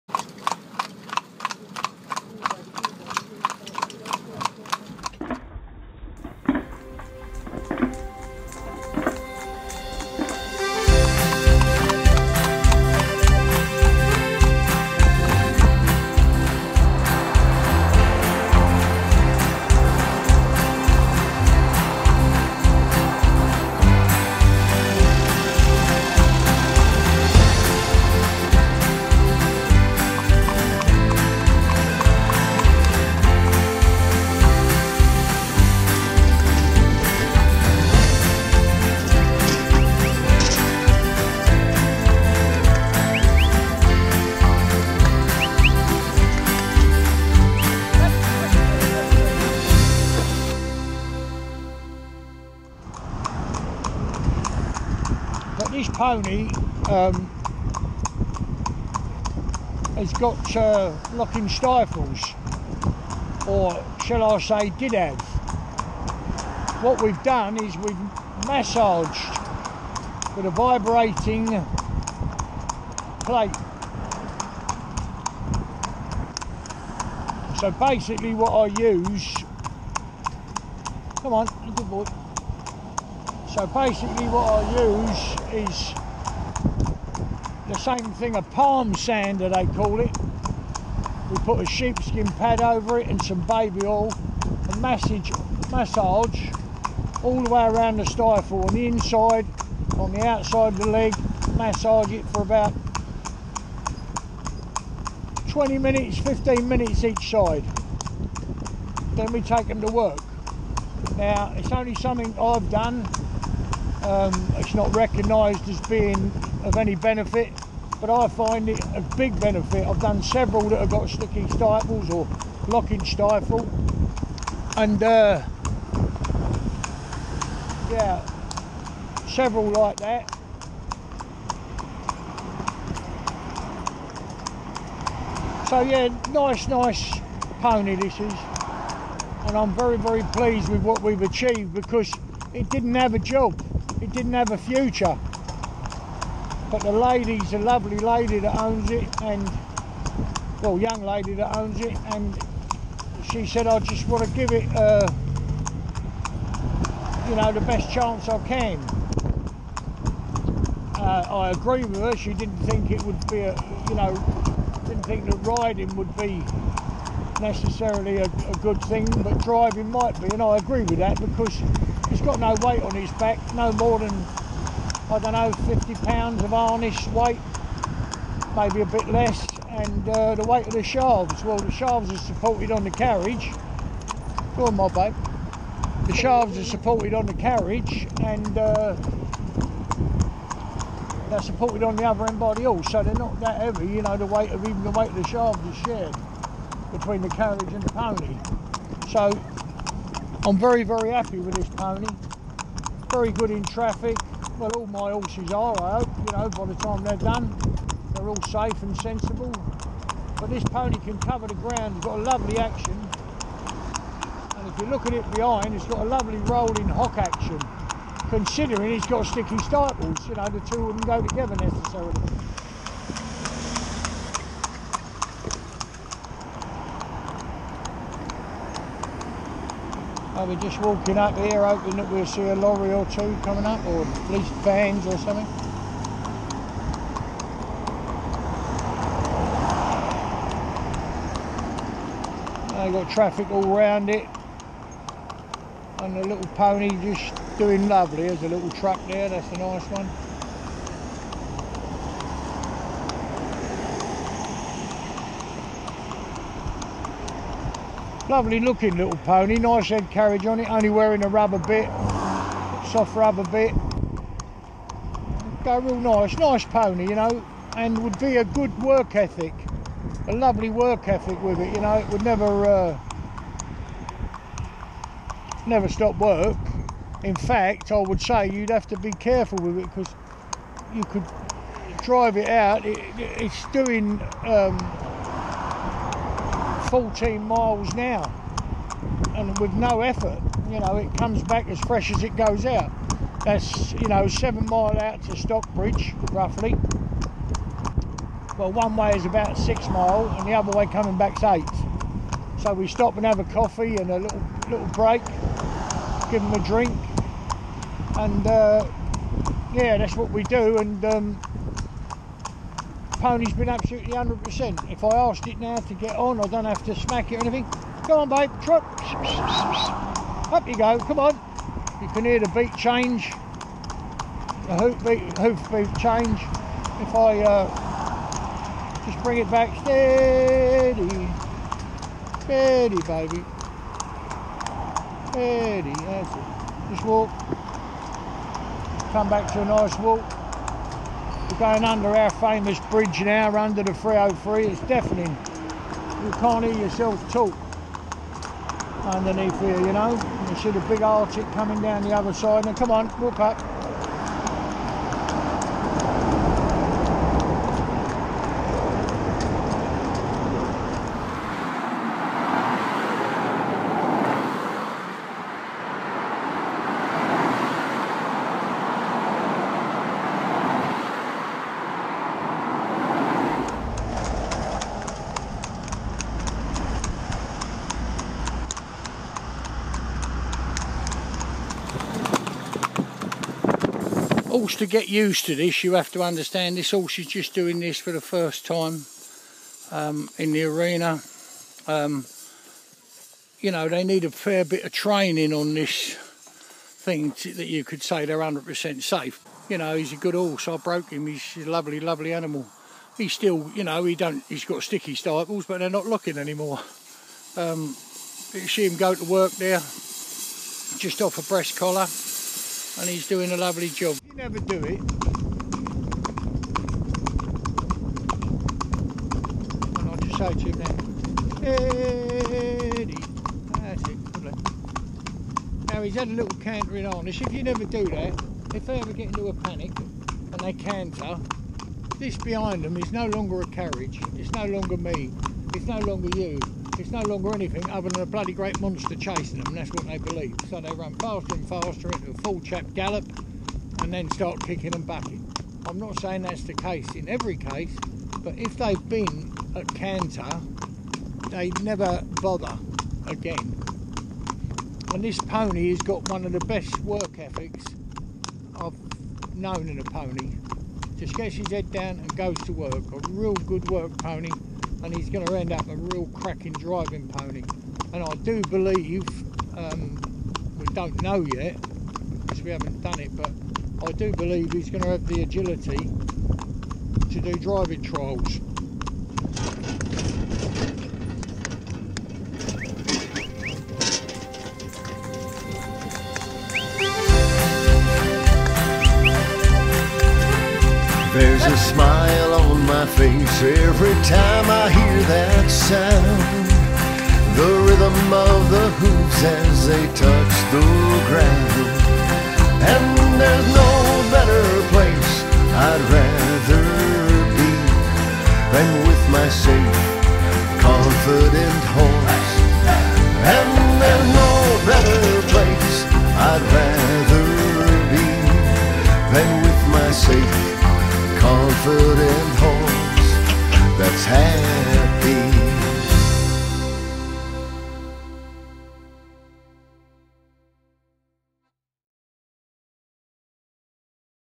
Training Toffee - a Pony with Locking Stifles
Meet Toffee, an unusual breed of Welsh pony known as the Carneddau. He has a history of locking stifles, which Barry treated with his own methods whilst Toffee was with us for training. As the weeks progressed, his regime of massage and exercise improved Toffee's stifles to the point where they were no longer 'locking', but merely occasionally 'sticky' under certain circumstances. However, despite working very hard, Toffee couldn't quite attain the lean physique of some of his more athletic classmates and thus earned the affectionate nickname on the yard of 'Sticky Toffee Pudding'!

Best of luck to him and his owner - who had second thoughts about delving into the adventure of competitive cones driving once she saw that Toffee might well be very good at it indeed if given the chance!

You can see more of Toffee (and his endless patience!) in our upcoming 'How to Harness' video series, as Barry shows how to take a harness and carriage from packed to pony for driving.